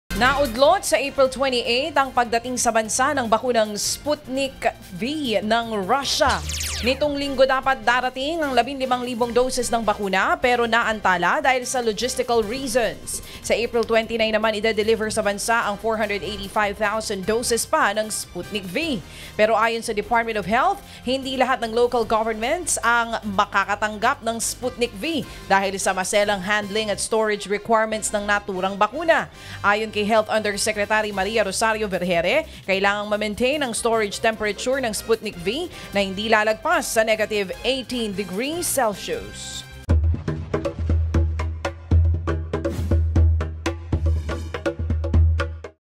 Pagdating ng Sputnik V, naantala
TedFailonandDJChaCha | Naudlot ang pagdating ng Sputnik V vaccines sa bansa.